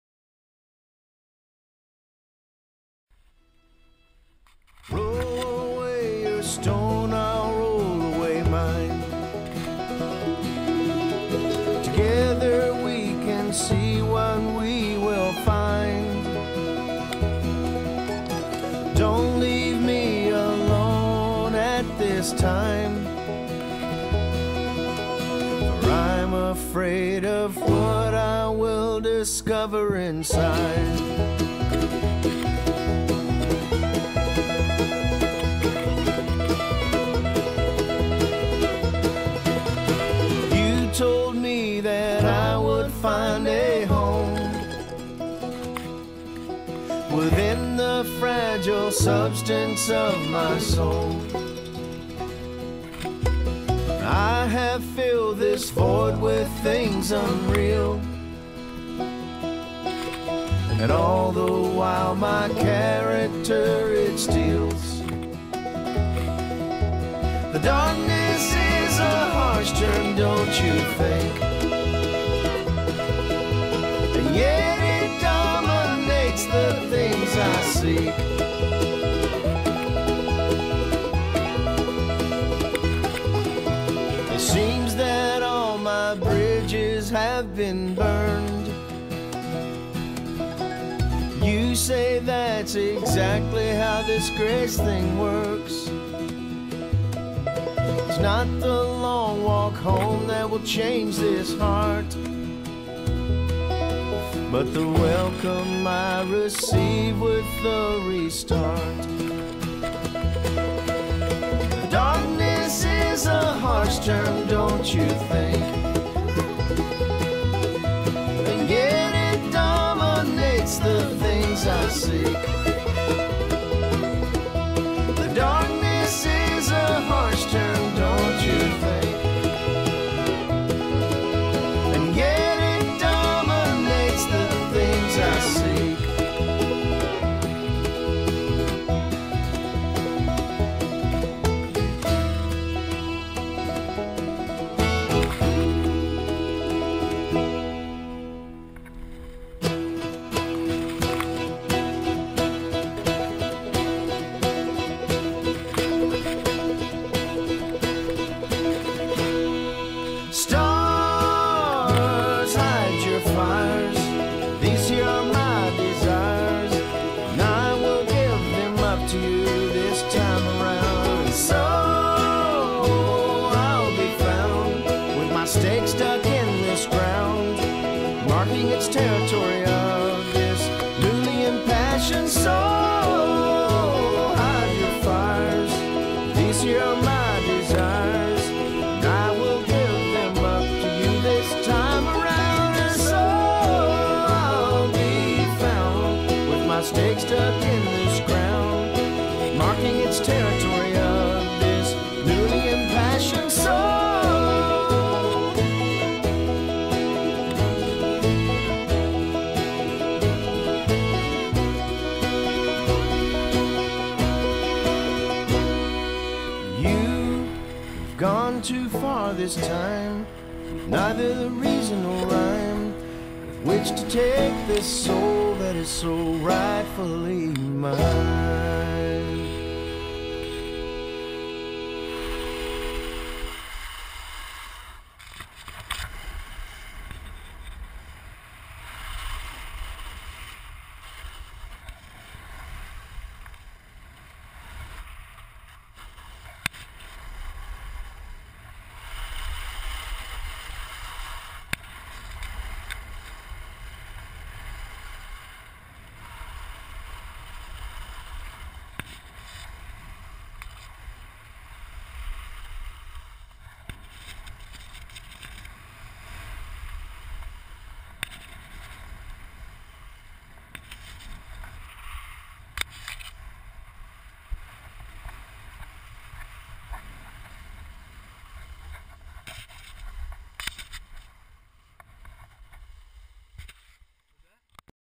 Sugarloaf 4 19
Run off of Sugarloaf Peak in the Bitterroot Mountains of Montana